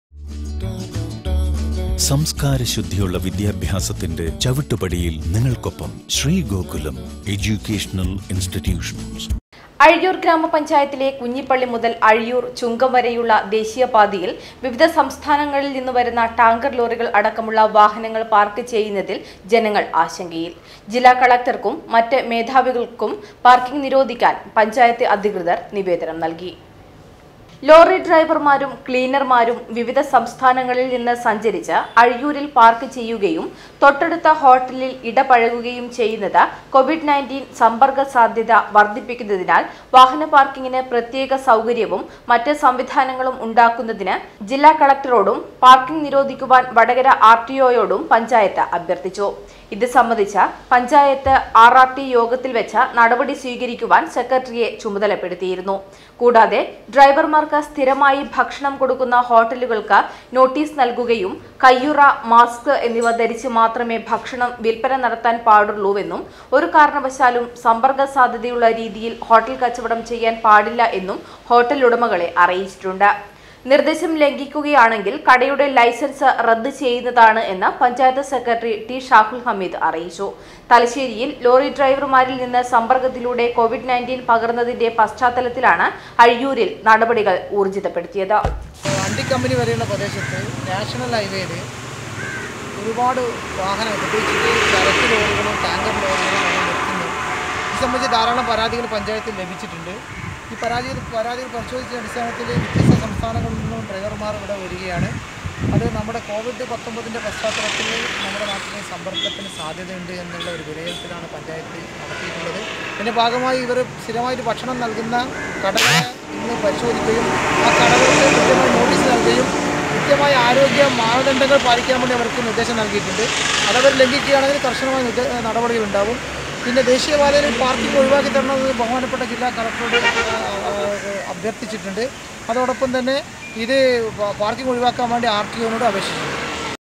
ടാങ്കർ ലോറികൾ അടക്കമുള്ള വാഹനങ്ങൾ പാർക്ക് ചെയ്യുന്നതിൽ ജനങ്ങൾ ആശങ്കയിൽ
അഴിയൂർ ഗ്രാമപഞ്ചായത്തിലെ കുഞ്ഞിപ്പള്ളി മുതൽ അഴിയൂർ ചുങ്കം വരെയുള്ള പഴയ ദേശീയ പാതയിൽ ടാങ്കർ ലോറികൾ അടക്കമുള്ള വാഹനങ്ങൾ പാർക്ക് ചെയ്യുന്നതിൽ ജനങ്ങൾ ആശങ്കയിൽ